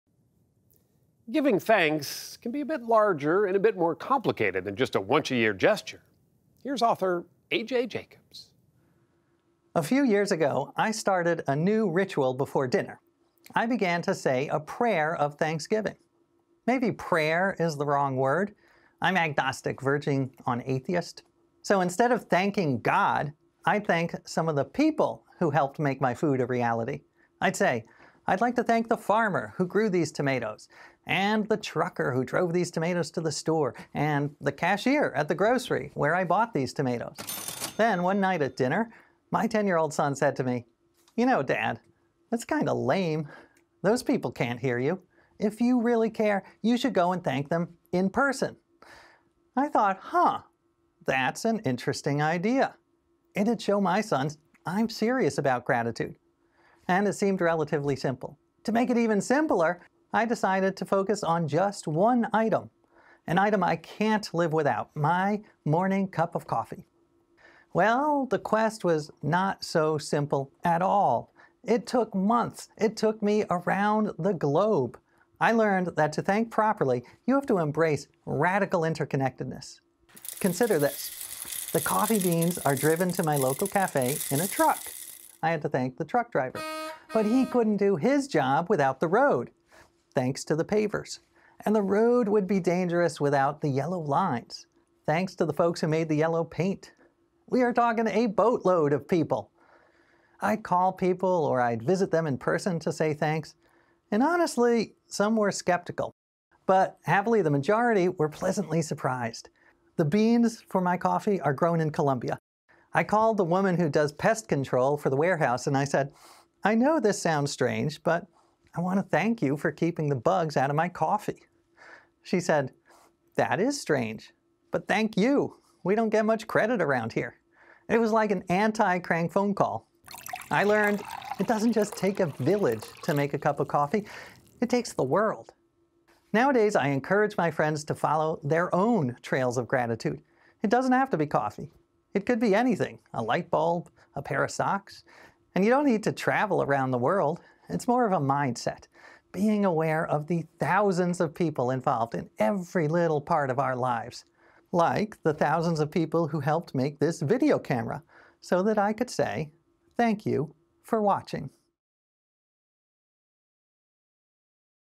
Giving thanks: A.J. Jacobs on his gratitude journey
Commentator and columnist A.J. Jacobs, author of "Thanks a Thousand: A Gratitude Journey," describes the delightful trouble he went through to thank everyone who helped create his morning cup of coffee.